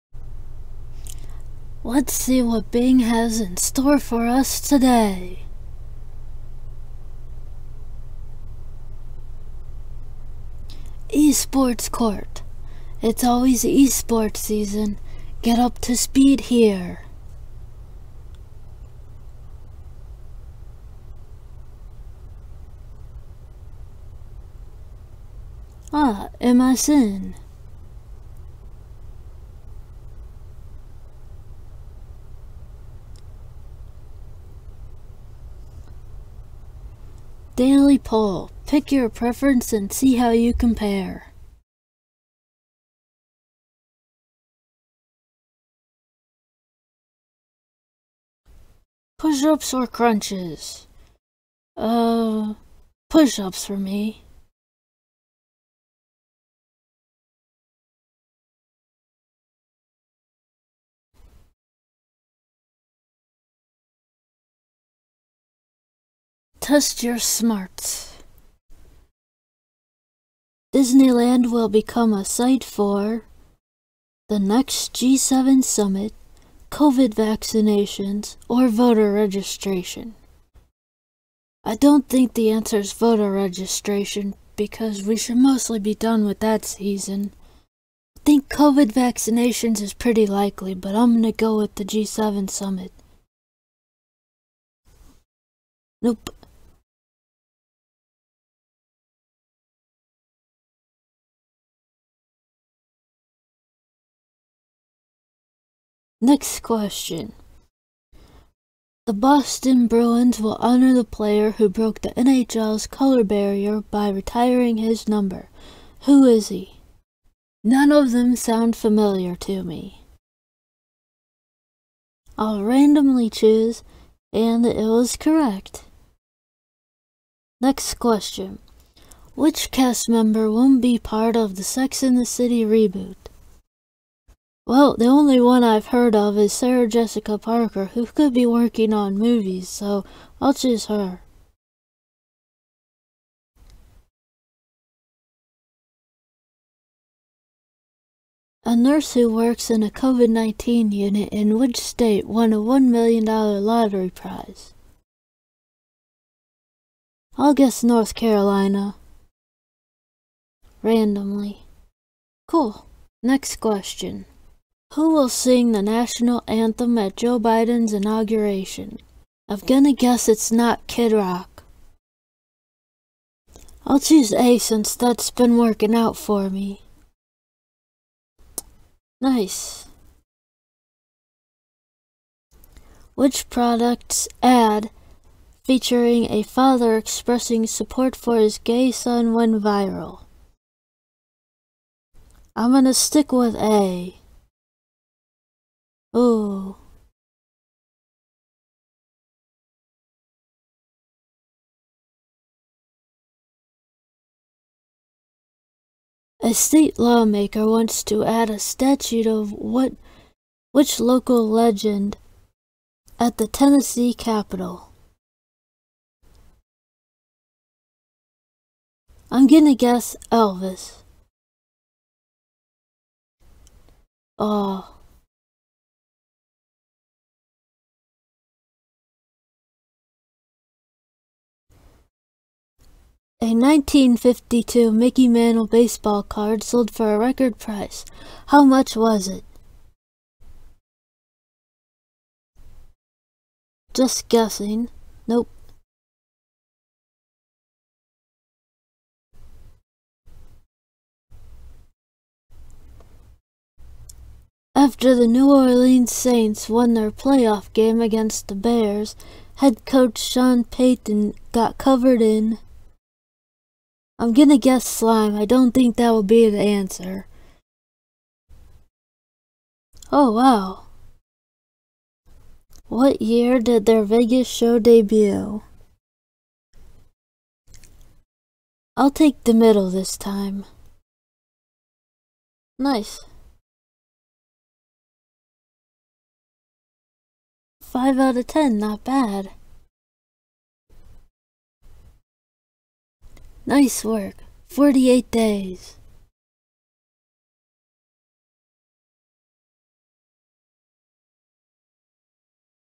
Bing News Quiz Week 2 of 2021
The Bing quiz for 15 January 2021 was about the week's news. I must have paid more attention to the news this week than I'd realized. Streak day 48.

I also covered the other tasks to keep my streak.

I apologize for the mispronunciations.